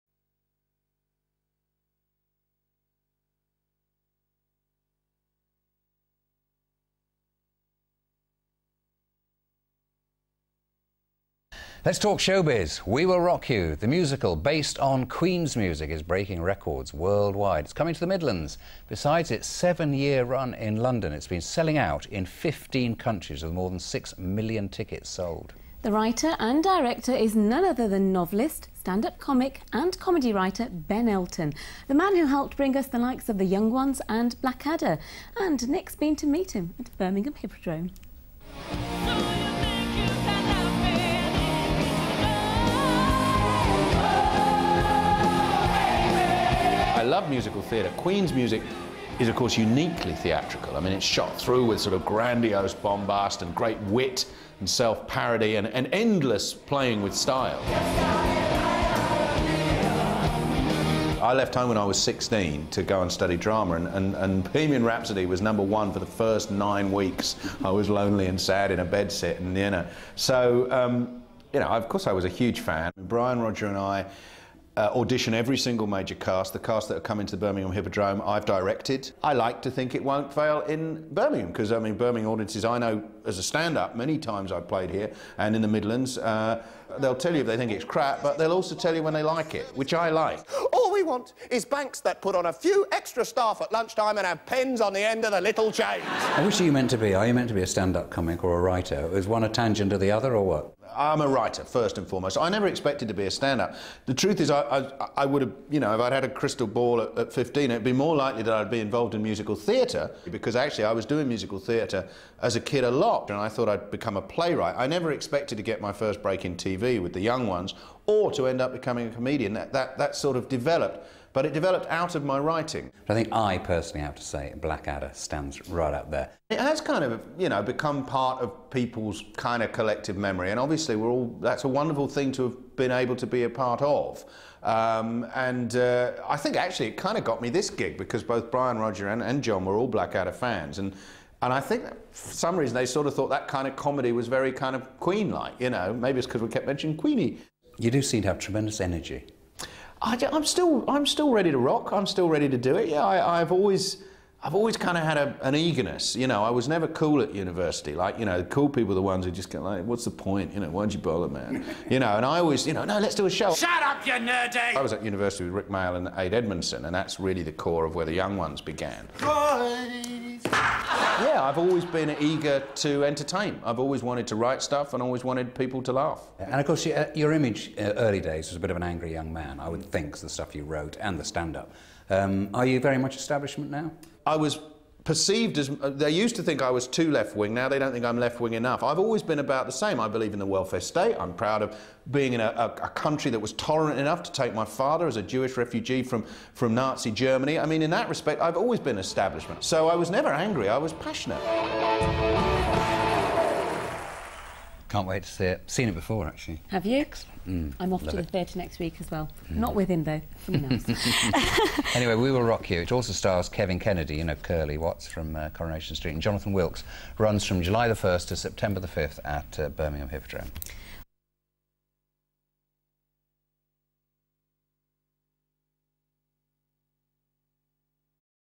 Nick Owen meets Ben Elton
Nick Owen from BBC Midlands Today meets comedian Ben Elton - the writer behind smash hit musical We Will Rock You.